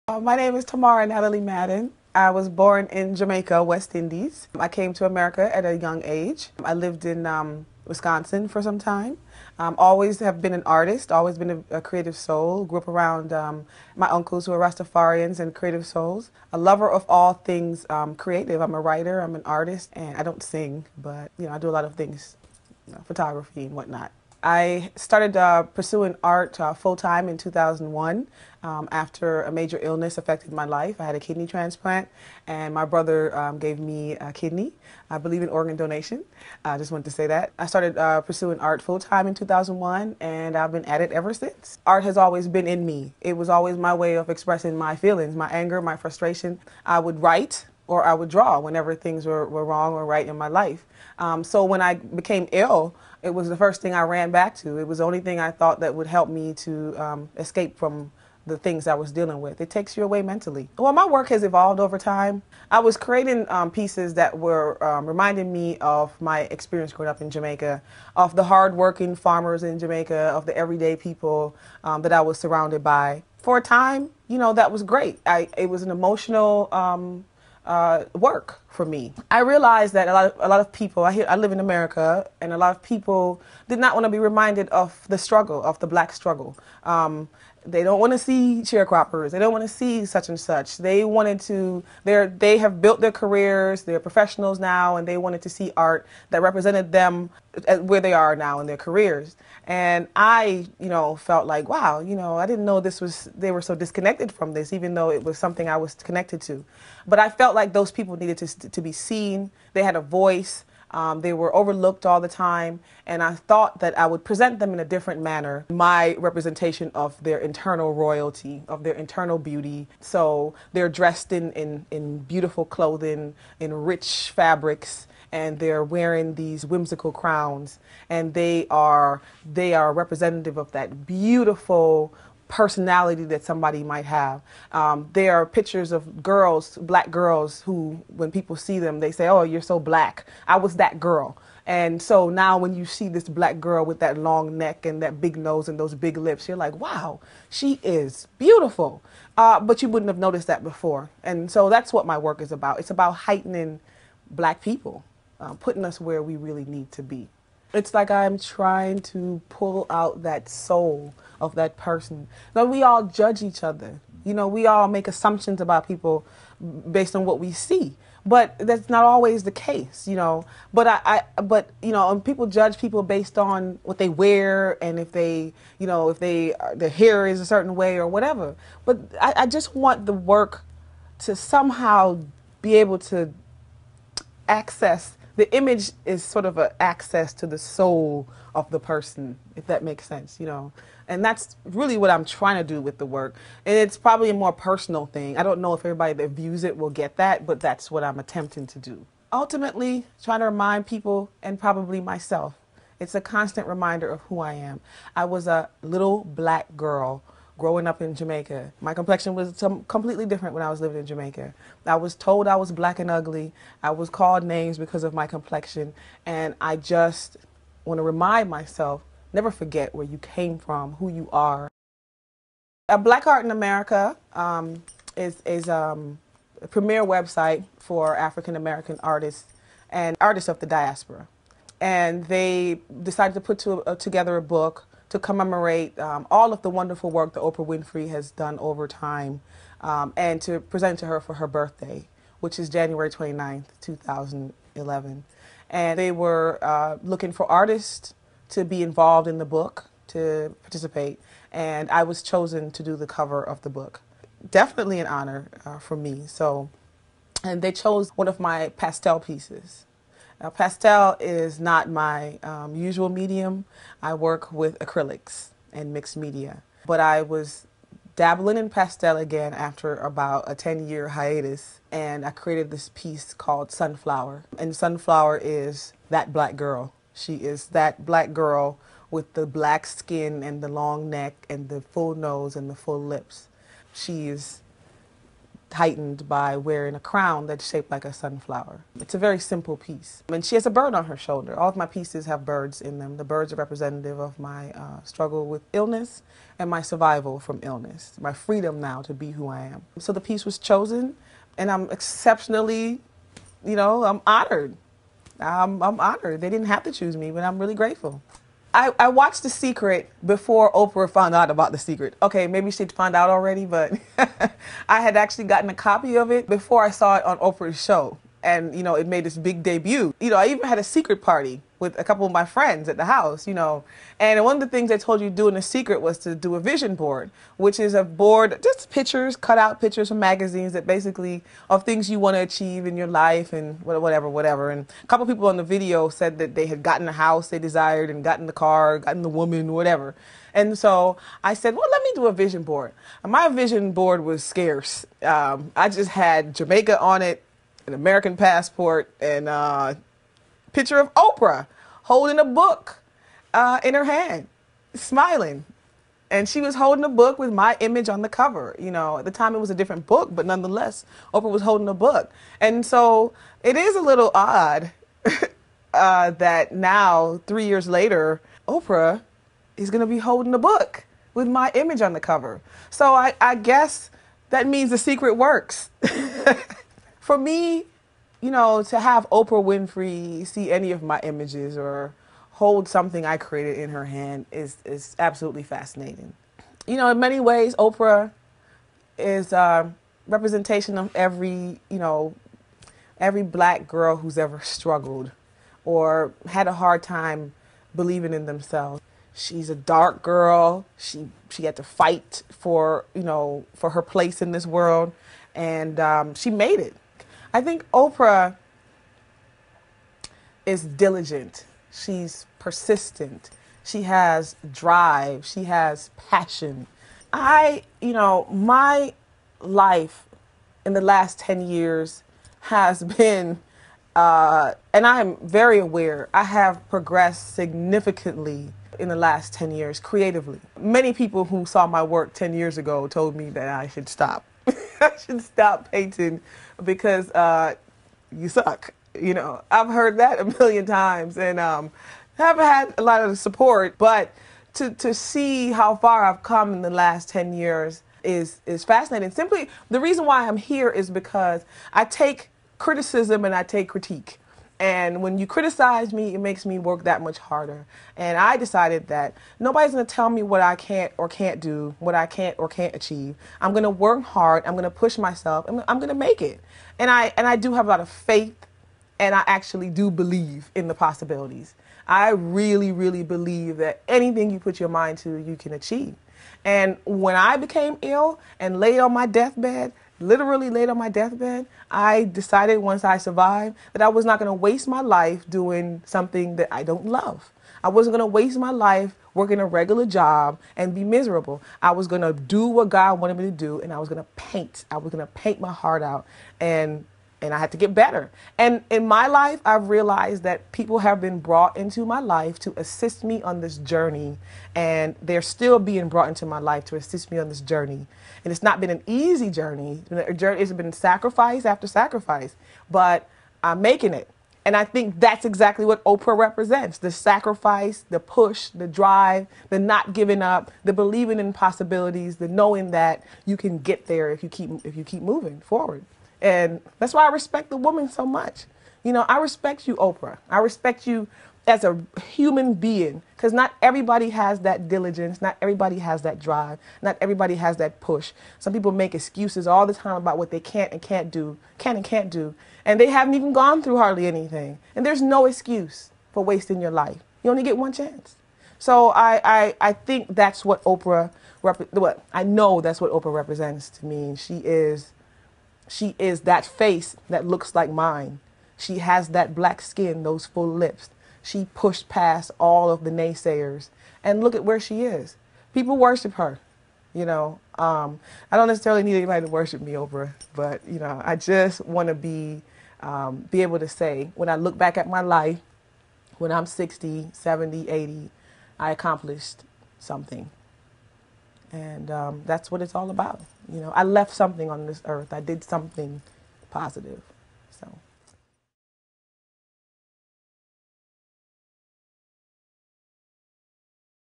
Tamara Natalie Madden interview
Artist Tamara Natalie Madden talks about her work being chosen to cover, "A time, A Season...a Commemorative Book to honor Oprah Winfrey.